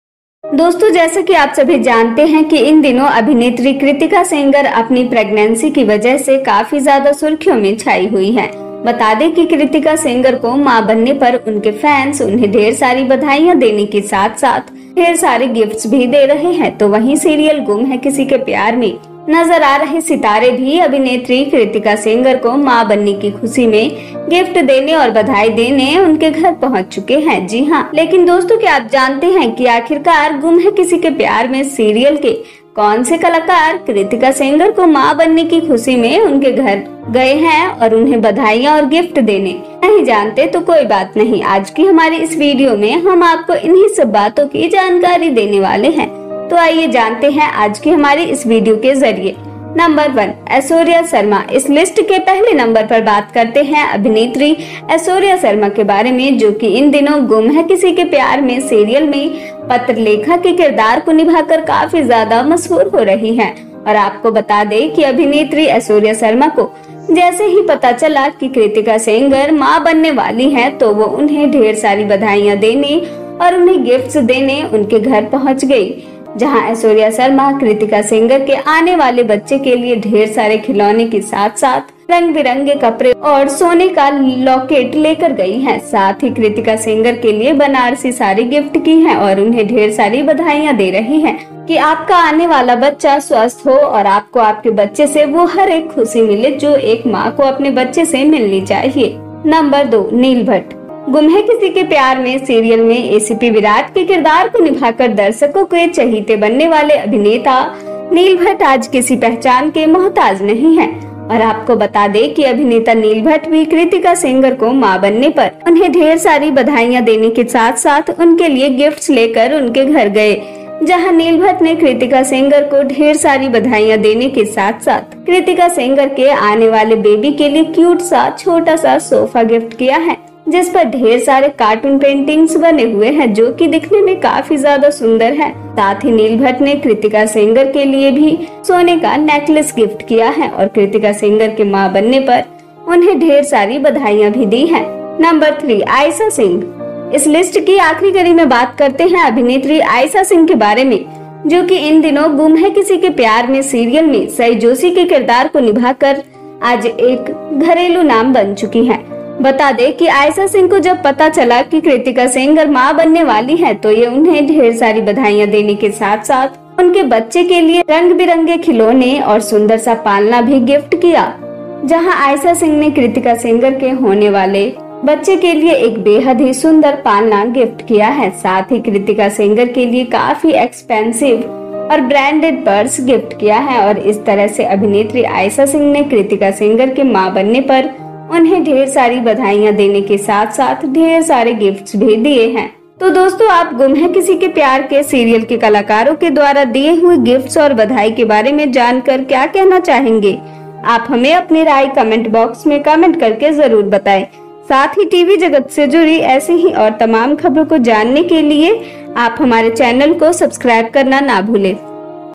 tv सीरियल की मशहुर अभिनेत्री कृतिका सेंगर को माँ बनने पर इन सितारों ने दिया एक्सपेंसिव गिफ्ट और बधाई
tv सीरियल की मशहुर अभिनेत्री कृतिका सेंगर को माँ बनने की खुशी में इन सितारों ने दिया एक्सपेंसिव गिफ्ट और बधाई नाम सुनकर चौंक जाएंगे